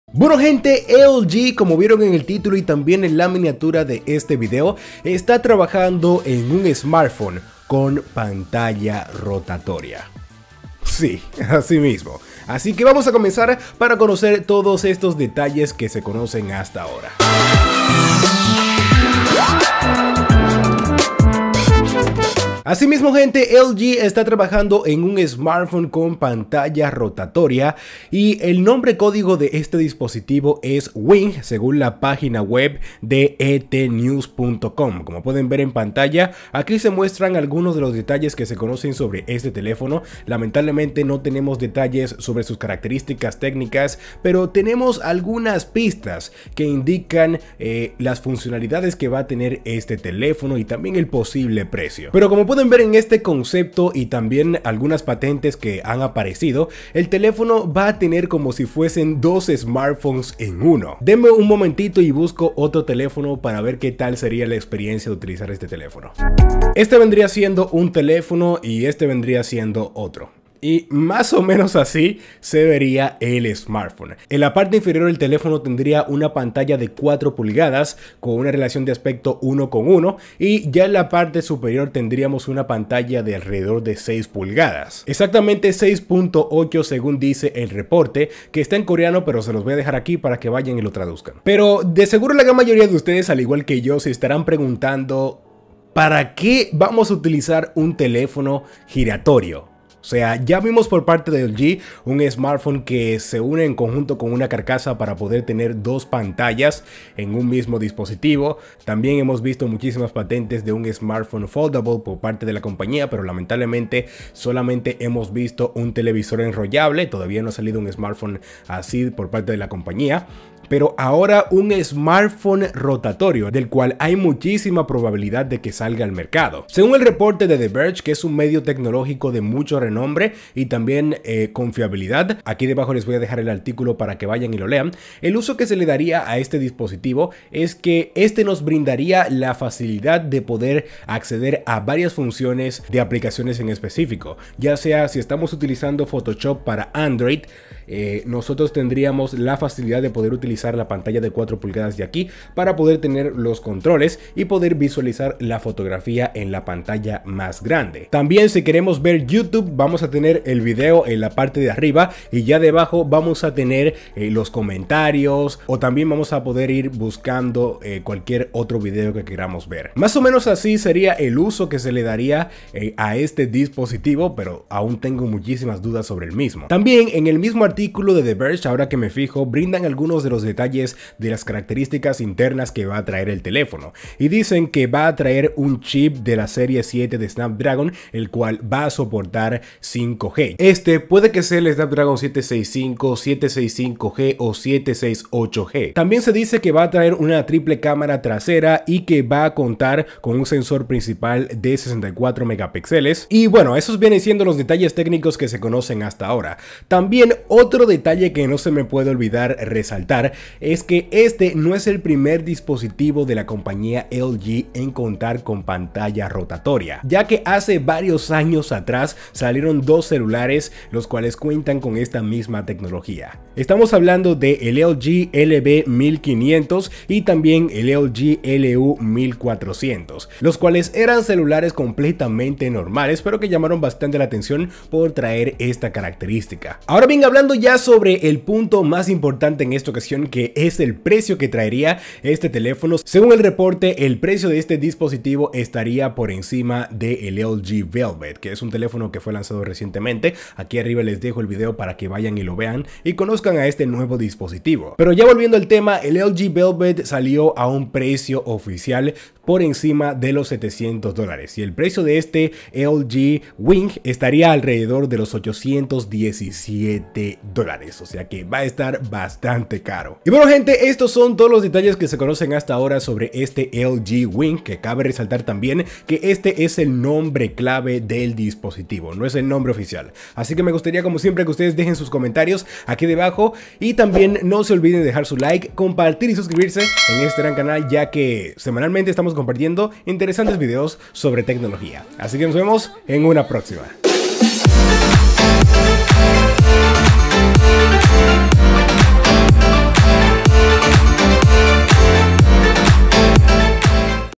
LG Wing ¡CELULAR CON PANTALLA GIRATORIA! ¿PERO QUE ES ESTO? | Todos los detalles en Español
LG Wing, Todos los rumores e informaciones que se conocen sobre este próximo smartphone con pantalla giratoria de LG.